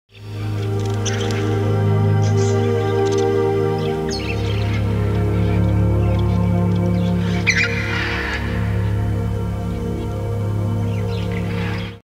IMAM / Soothing music #7 / The sounds of sunrise /
Play this video when you are overworked, tired, or just need a moral reboot. This melody will help to relax all your nerve endings, tone all the senses and you can easily return to any process with renewed vigor!♥